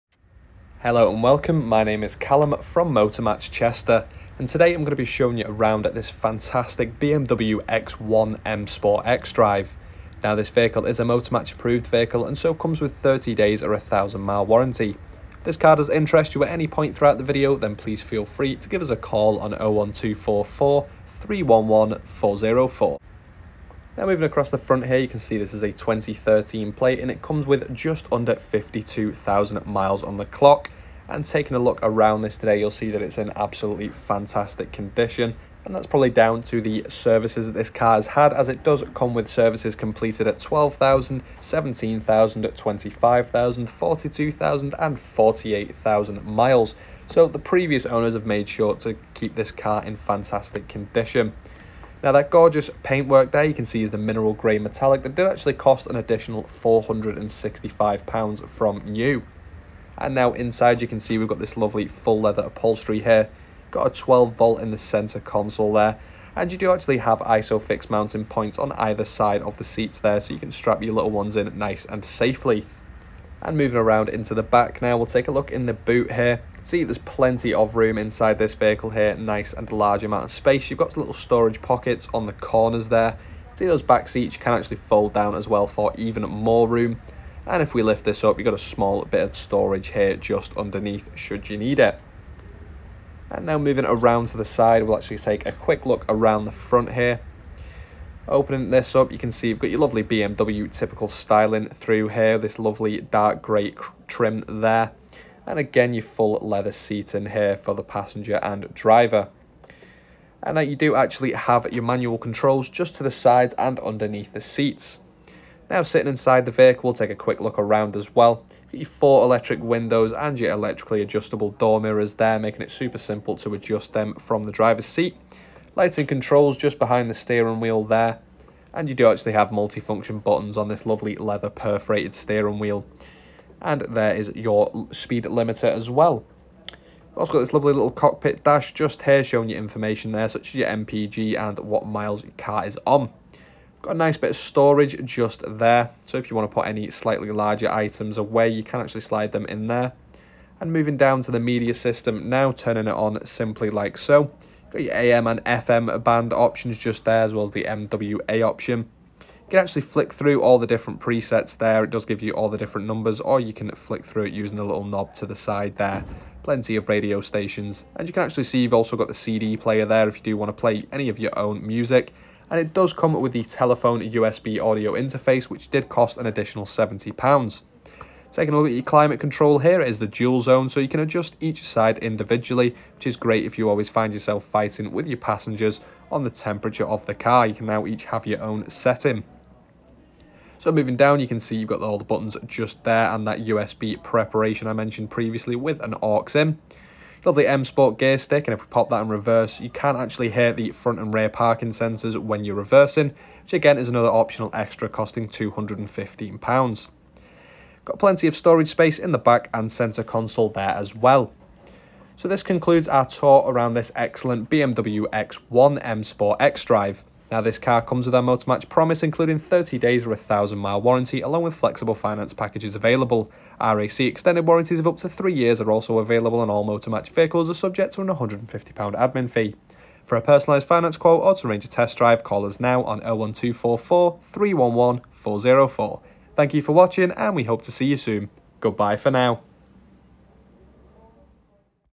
Used BMW X1 2.0 20d M Sport xDrive at Chester | Motor Match Used Cars for Sale
Just into our stock, this 2013 BMW X1 2.0 20d M Sport xDrive in Grey, is now available for sale from Chester.

Reg number: YK13FHZ
Fuel: Diesel
Transmission: Manual
Mileage: 51,983

Time Stamps
Introduction 0:00
Exterior Front Tour 0:23
Cockpit Tour 1:55
Contact Information 3:32